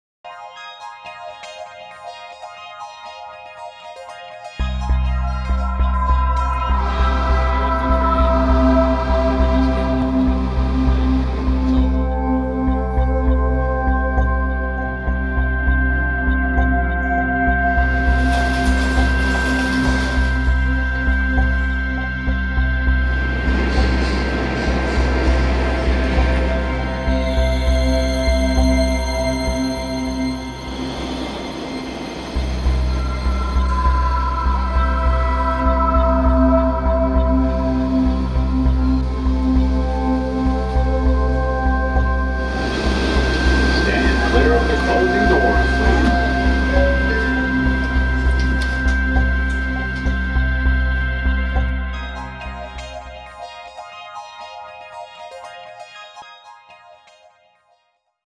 New Subway Station NYC
The almost finished, new Bleecker Street subway station features changing fluorescent lights, and the convenience of not having to exit and re-enter the station from the street.  It will be finished soon!